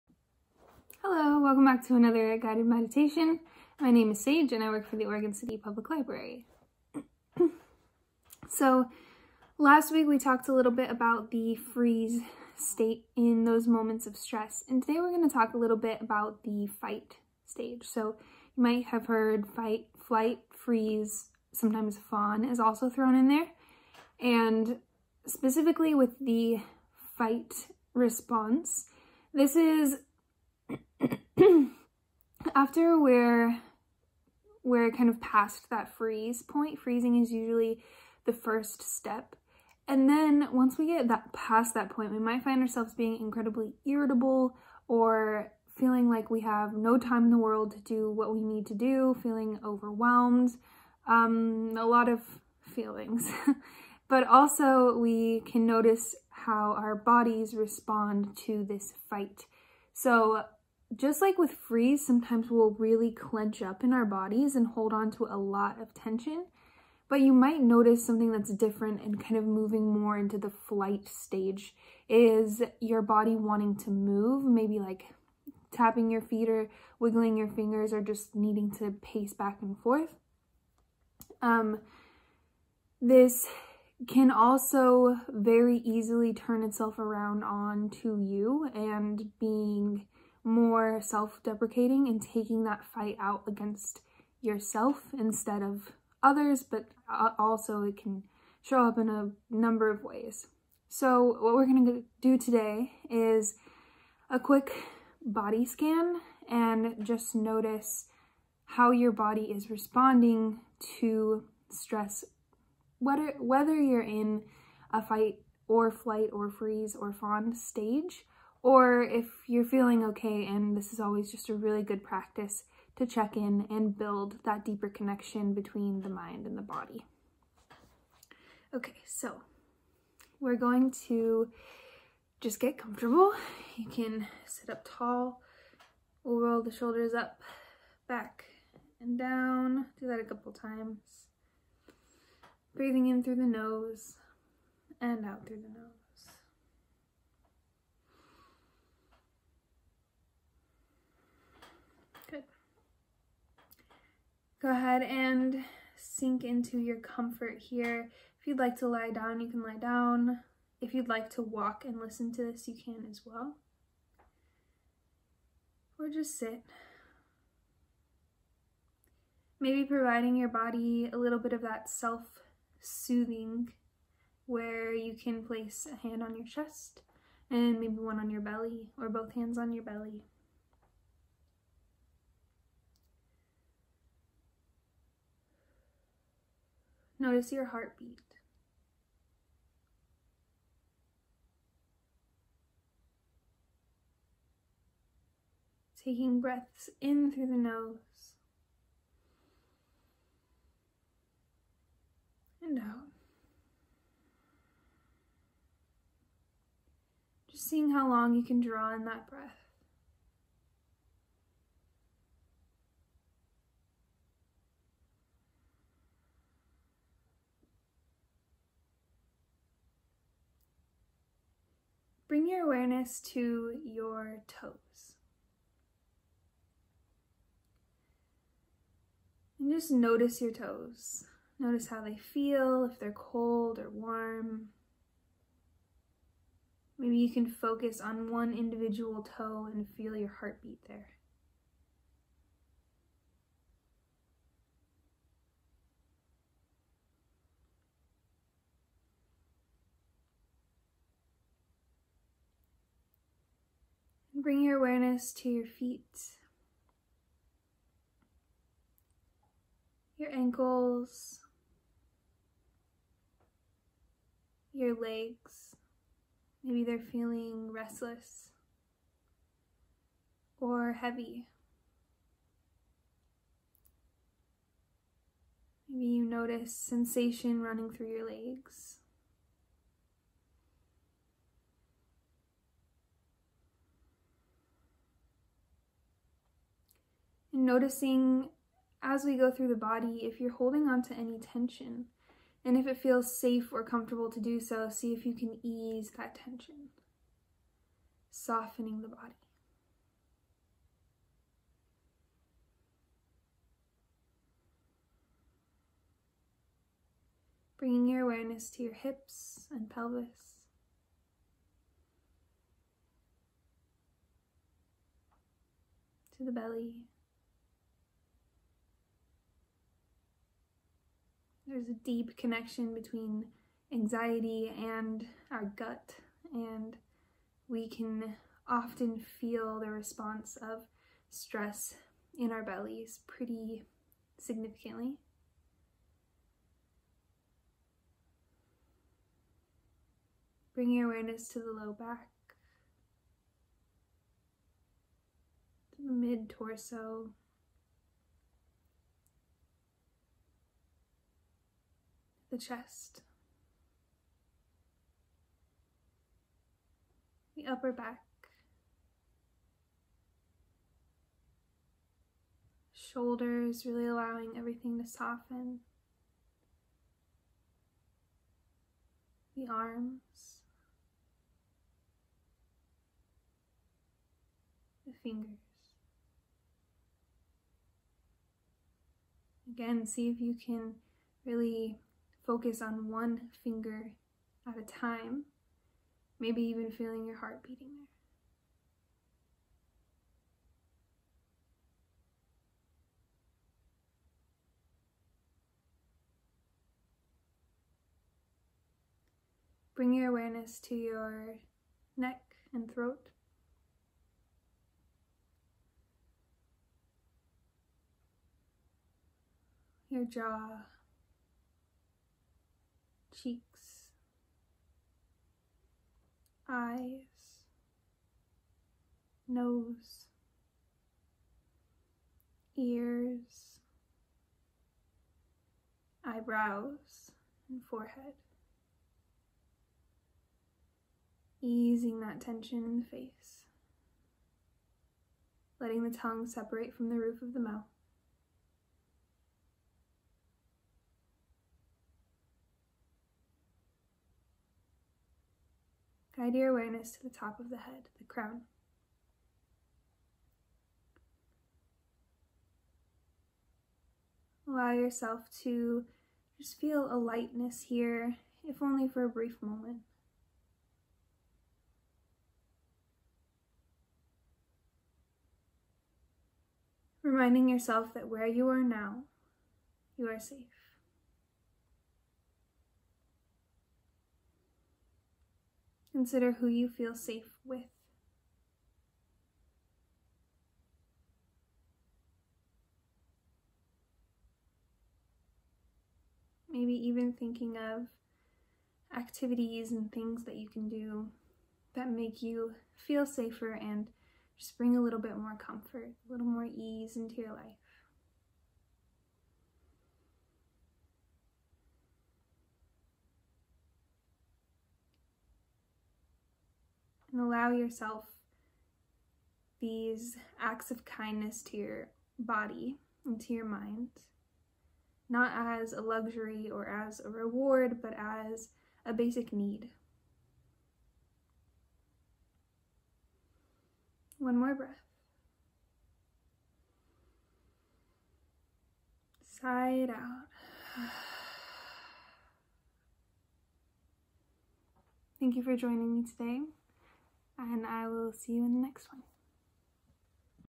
Guided Meditation with Sage — Body Scan for the “Fight” Response
Mindfulness Mondays: Guided Meditations with Sage
Start your work week with a guided meditation. Meditation reduces stress, helps control anxiety, and promotes emotional health. This week's meditation: Body Scan for the “Fight” Response.